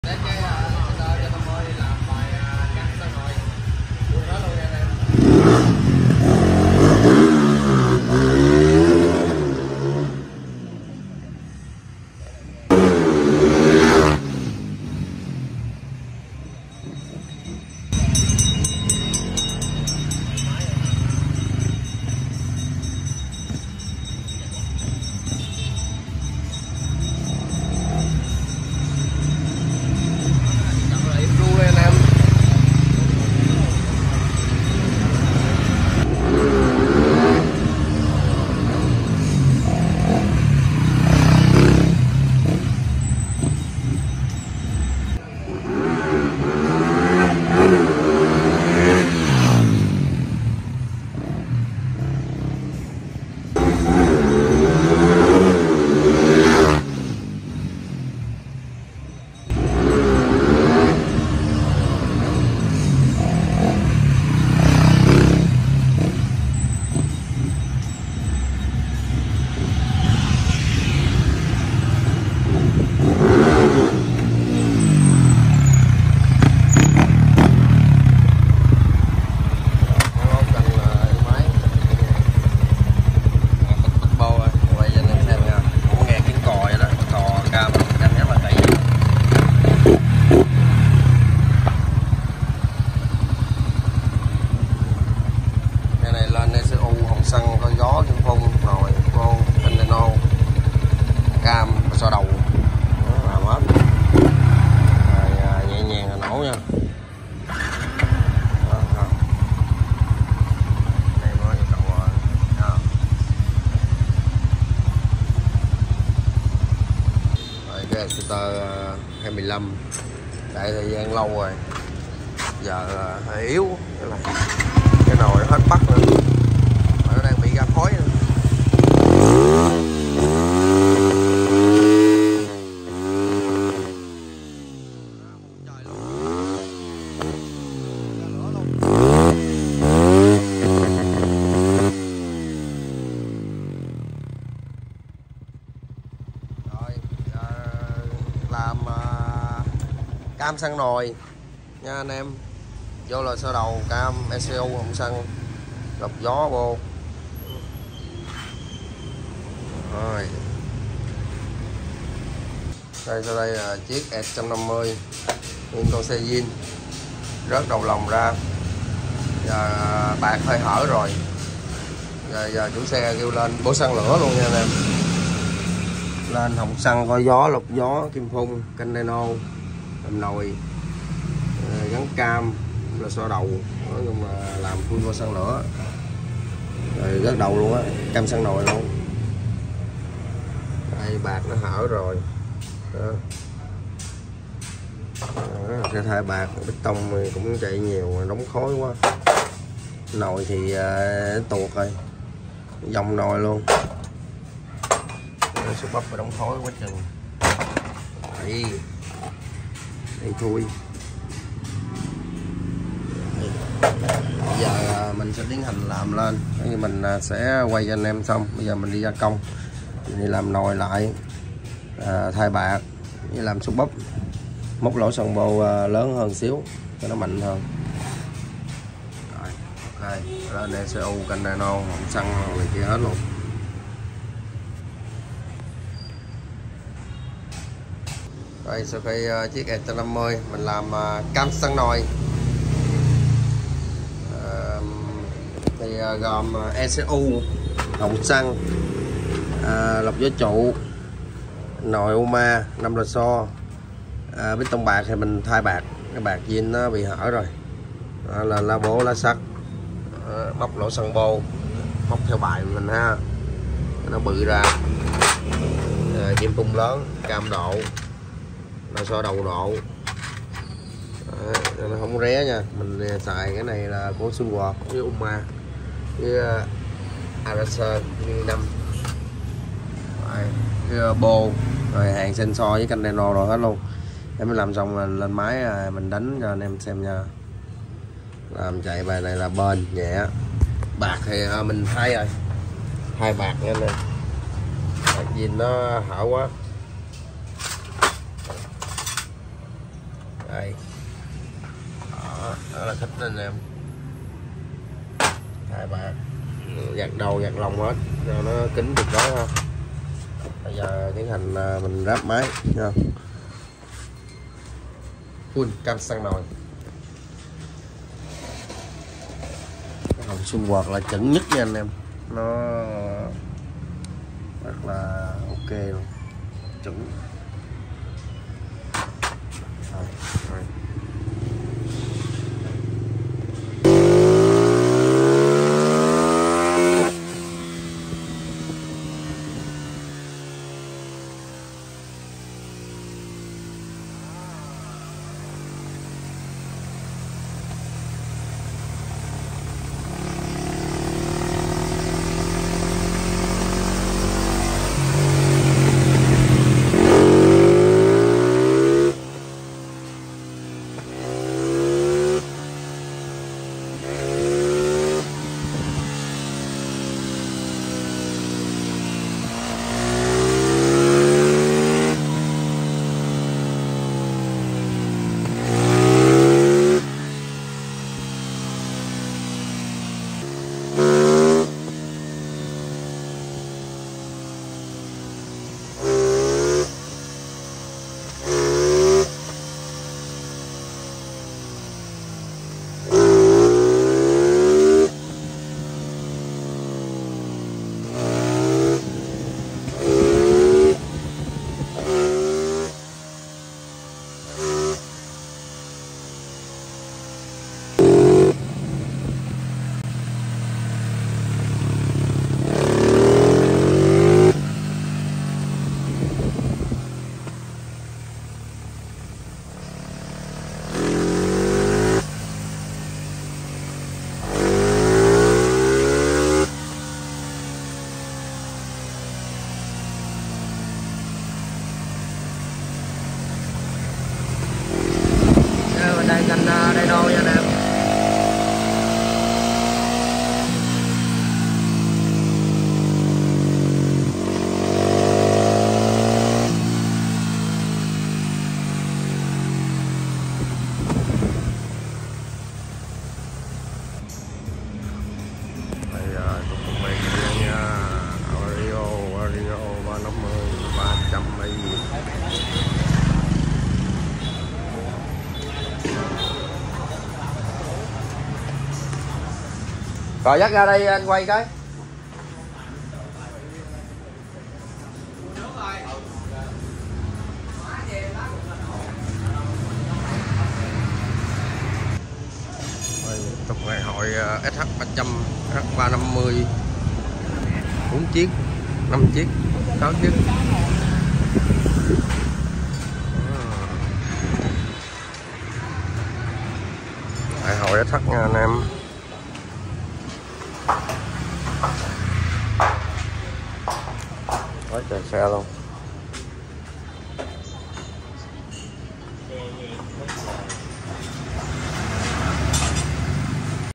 Exciter 150i độ Cam xăng nồi thay bạc gắn ECU Họng Xăng Lớn Lọc Gió Trụ nồi UMA Pô Móc Canh Dyno !
Exciter 150i độ Cam xăng nồi thay bạc gắn ECU Họng Xăng Lớn Lọc Gió Trụ nồi UMA Pô Móc Theo Bài Canh Dyno Full hết xe chạy mạnh bốc êm ái tại TBK Team 147 Tân Khai P4 Q11 Bảo Hello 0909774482 Thanks Mn